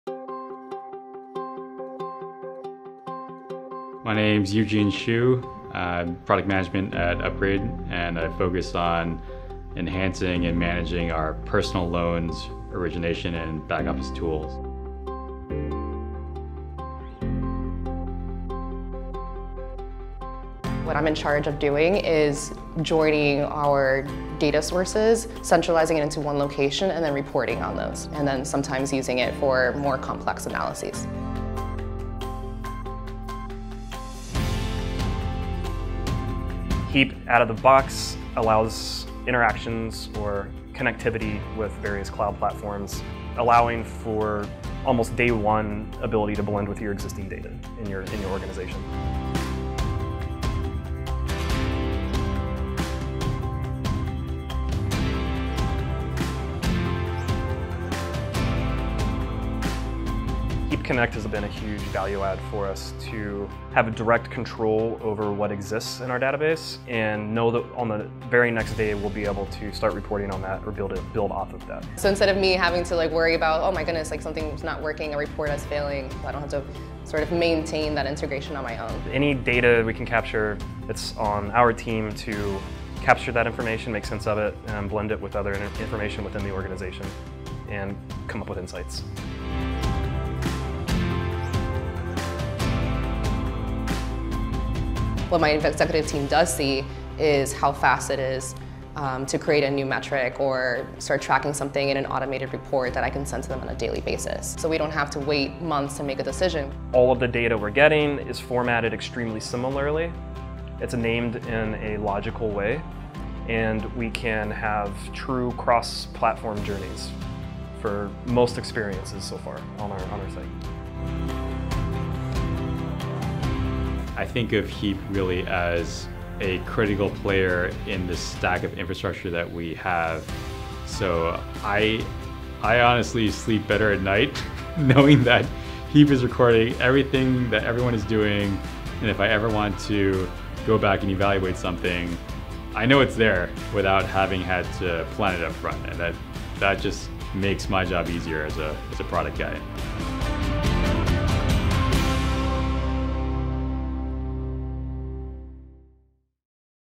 Heap Connect
Heap Connect is the smarter way to bring behavioral and product data to your data warehouse. Our managed ETL automatically integrates with Snowflake, Redshift, or BigQuery to bring Heap's autocaptured customer data to your data team.

To learn more, visit Heap.io.